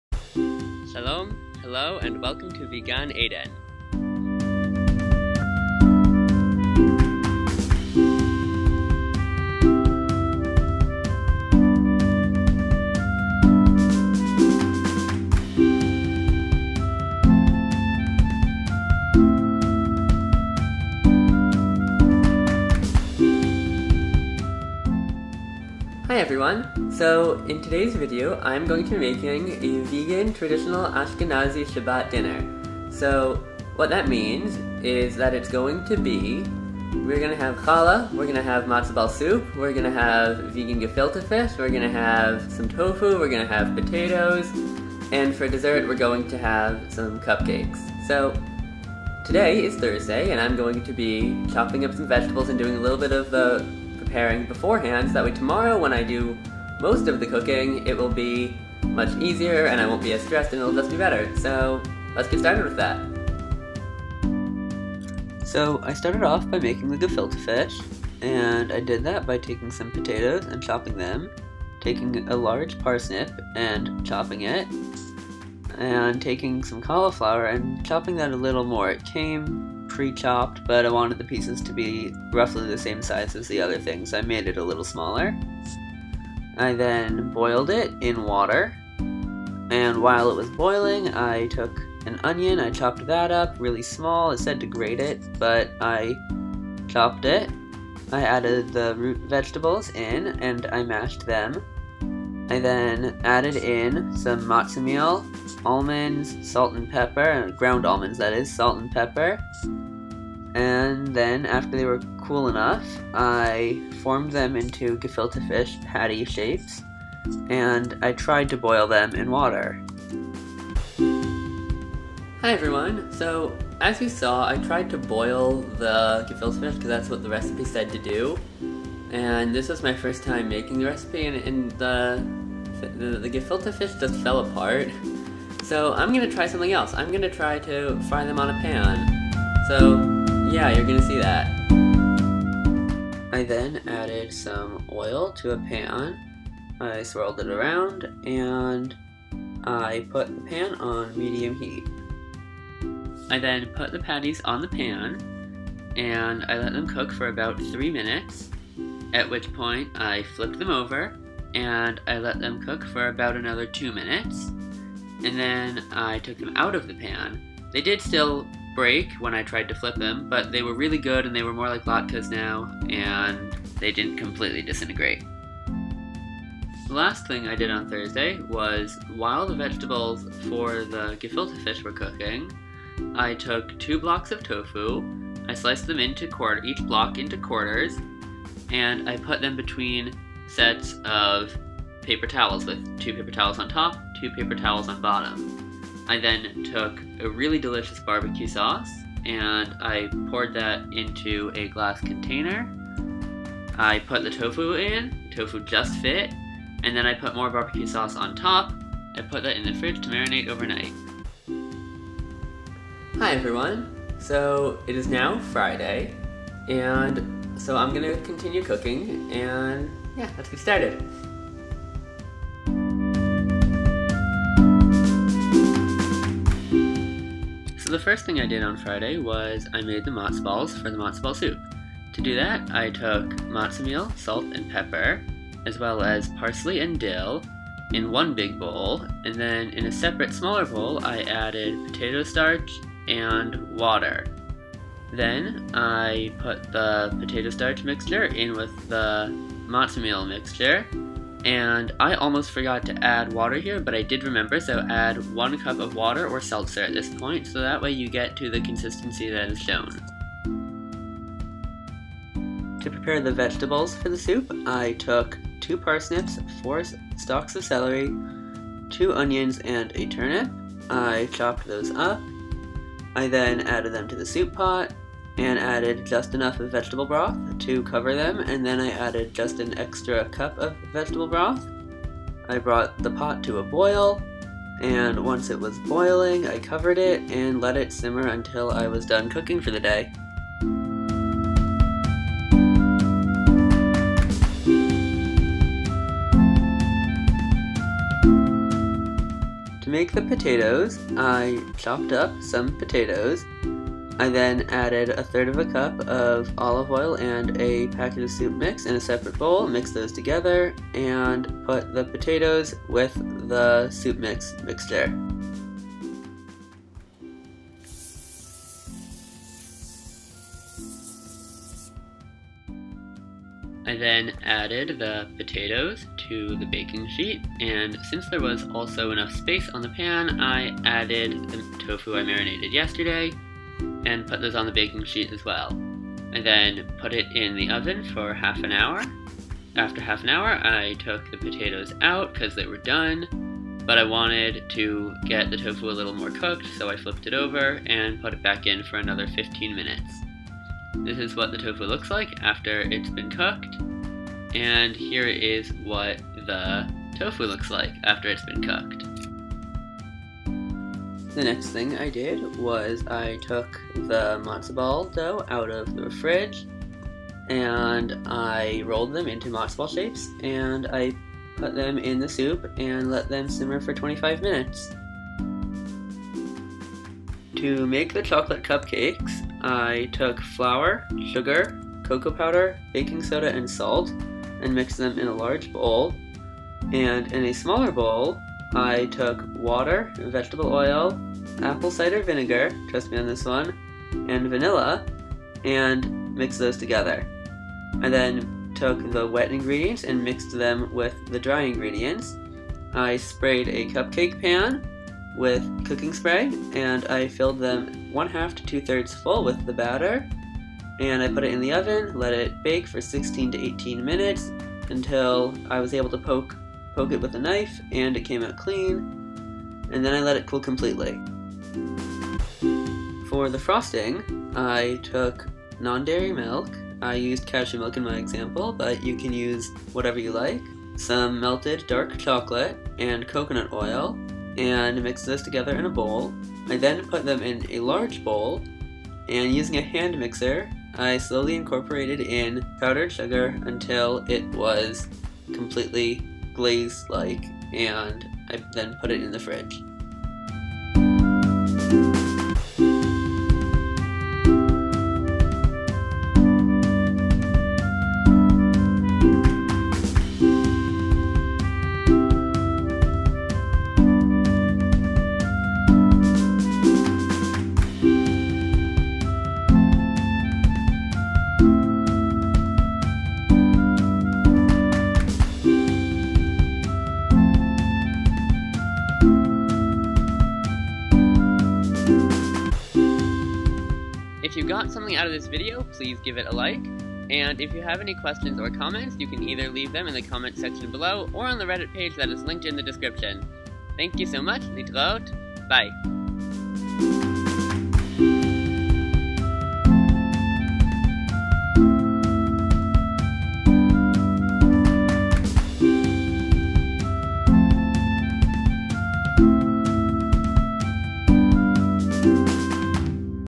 Seven Recipes for a Delicious Vegan Shabbat (Shabbos) Dinner!
In this video, I make a vegan version of a traditional Ashkenazi (Eastern European) Jewish shabbat dinner. It has gefilte fish, tofu, potatoes, challah, matsah (matzah) ball soup, and chocolate cupcakes. I made it for my family Friday night, and it was delicious! We all loved it!

This is my first recipe video, so let me know in the comments or on the Reddit page below if you have any suggestions on how to improve this style of video.

Challah dough recipe (adapted from "Kosher by Design: Teens and 20-Somethings" by Susie Fishbein):

INGREDIENTS
1 warm cup water
1tsp salt
3tbsp sugar
2tbsp canola oil
4 cups flour
1tbsp active dry yeast

DIRECTIONS
Set to dough setting in bread machine, or-
1. Mix the yeast, 1tbsp sugar, and water in a glass bowl
2. Using the dough hook attachment of a stand mixer (or whisk by hand), combine the rest of the sugar, water, salt, and oil on medium-low speed.
3. Add the yeast mixture, and knead for 4-5 minutes on medium speed.
4. Cover with a towel and let rise for 1.5 hours
5. Follow instructions provided in video.